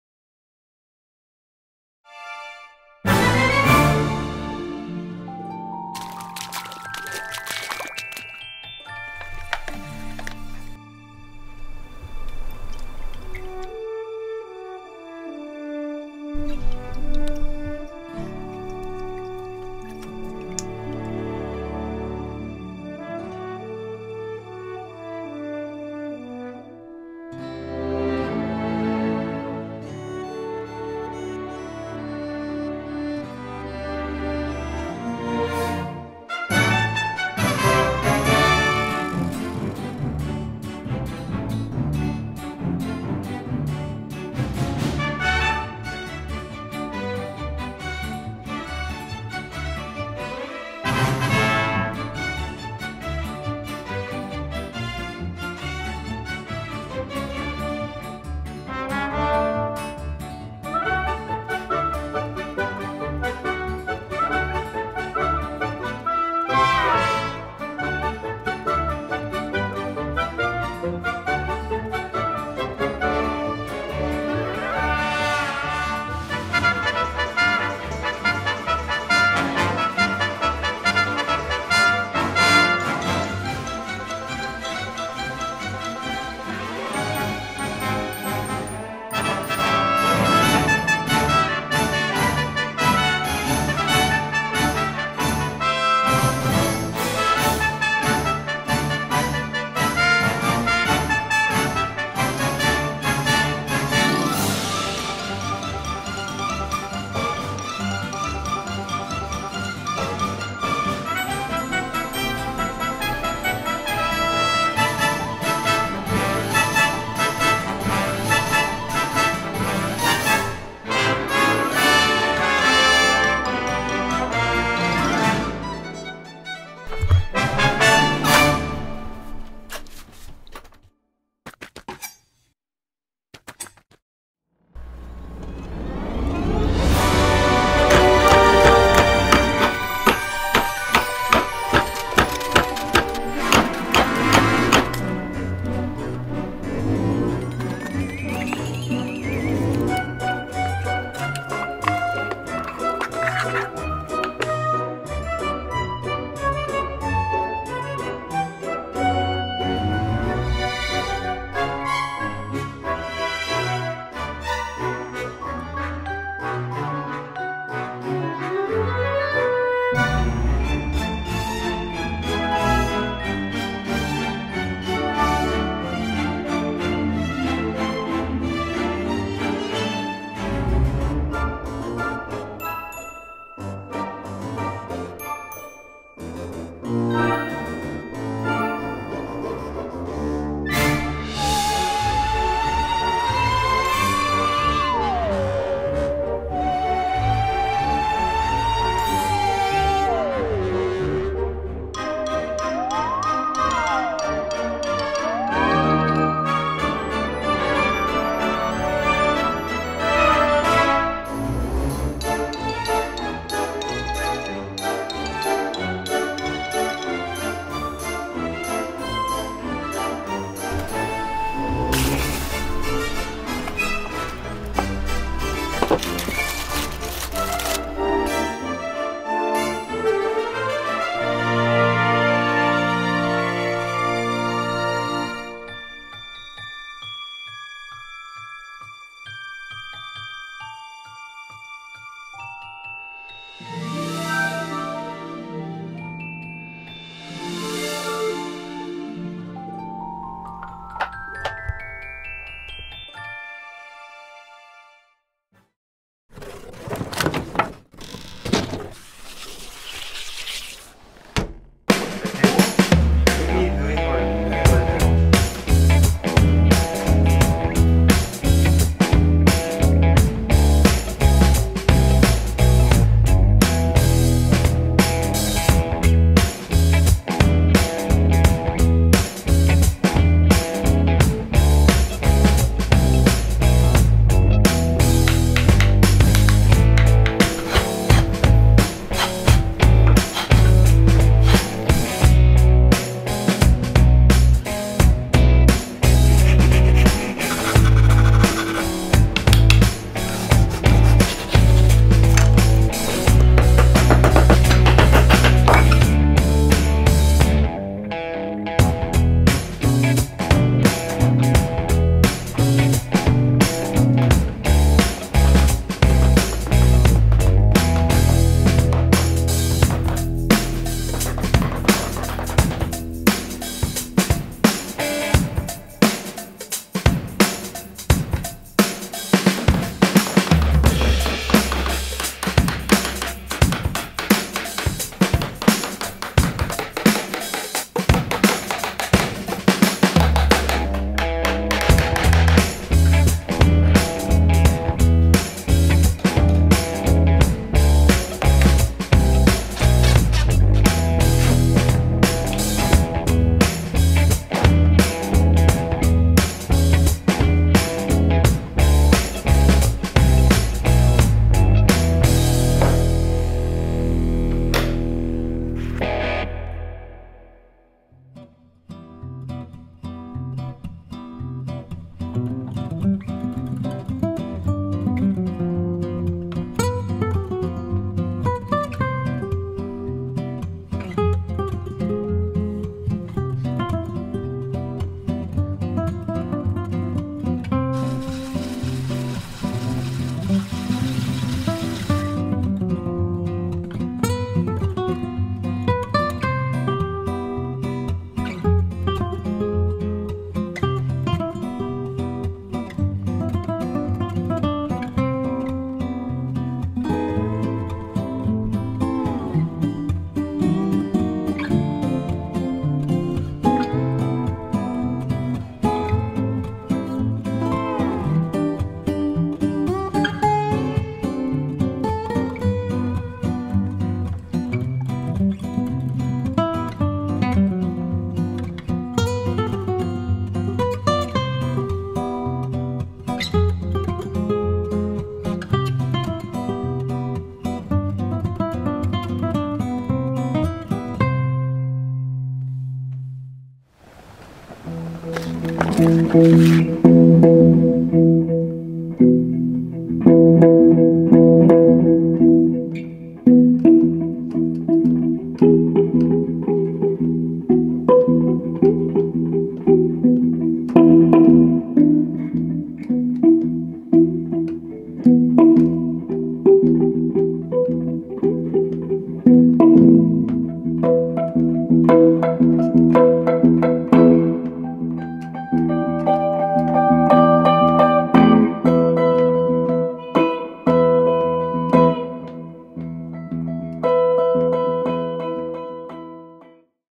[Guitar Making]Natural Lacquer Finishing & Guitar Case Making
We finished the guitar with a traditional Korean technique. It is painted with Natural Lacquer extracted from 'Ott Tree', and is a traditional 'Na-jeon' technique using mother-of-pearl. And the guitar case also worked with traditional masters. It was a tremendous experience.